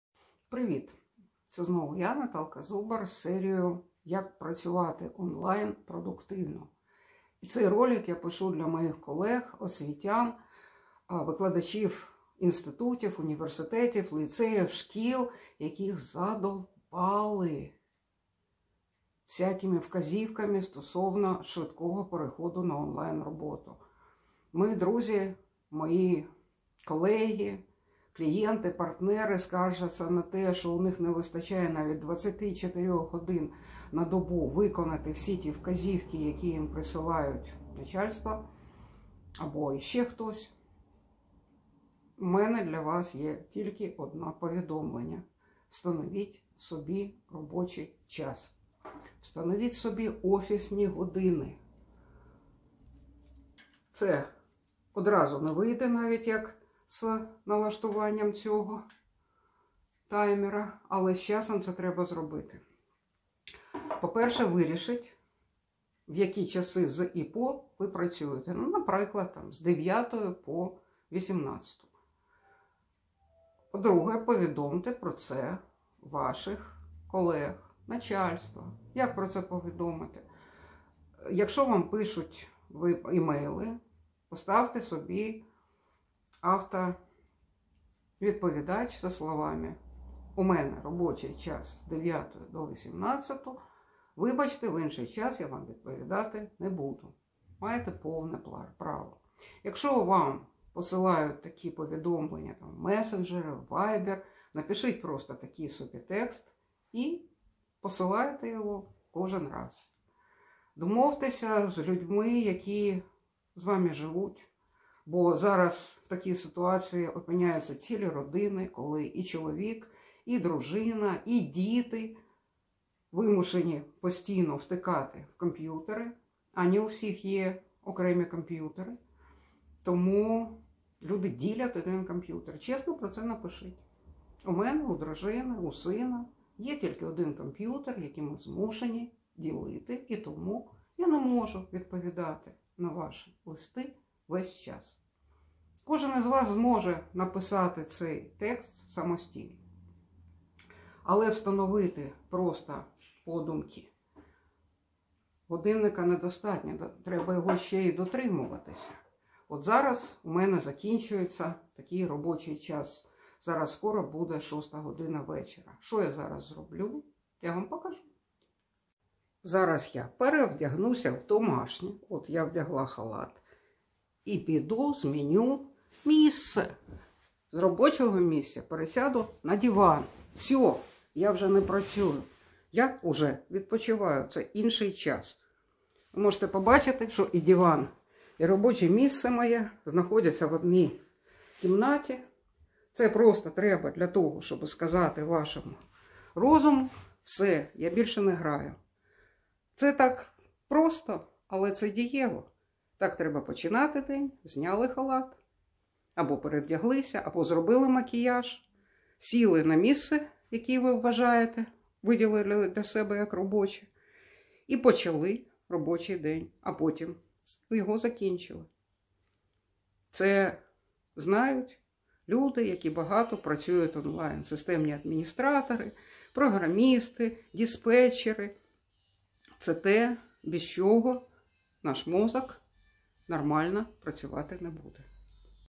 Життя Онлайн. Як встановити собі робочі часи онлайн
Через роботу онлайн перестало вистачати 24 годин на добу? Як організувати робочий час онлайн. Послання до друзів, особливо освітян, від Наталії Зубар